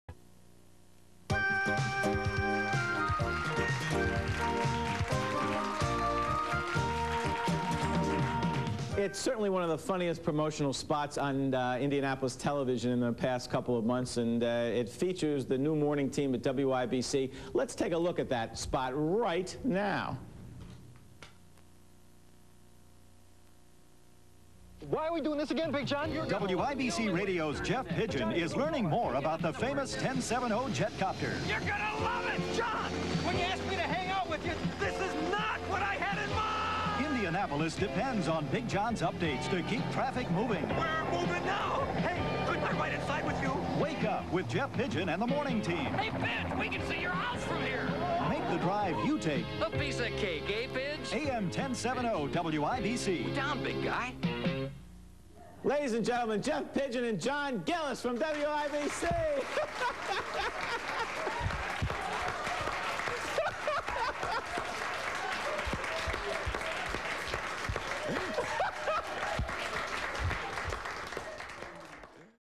Jeff Pigeon Big John Gillis Cake DIck Woolfsie
A clip from a show Dick Woolfsie hosted, that featured Jeff Pigeon and Big John GIllis and a helicopter trip John took Jeff on :)